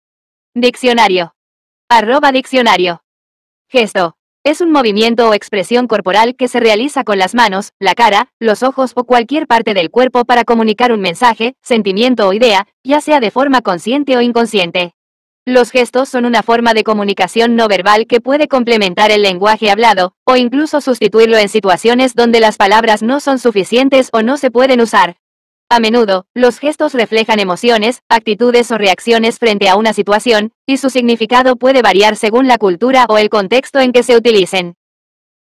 Gesto Significado En Español
Gesto: Es un movimiento o expresión corporal que se realiza con las manos, la cara, los ojos o cualquier parte del cuerpo para comunicar un mensaje, sentimiento o idea, ya sea de forma consciente o inconsciente. Los gestos son una forma de comunicación no verbal que puede complementar el lenguaje hablado, o incluso sustituirlo en situaciones donde las palabras no son suficientes o no se pueden usar. A menudo, los gestos reflejan emociones, actitudes o reacciones frente a una situación, y su significado puede variar según la cultura o el contexto en que se utilicen.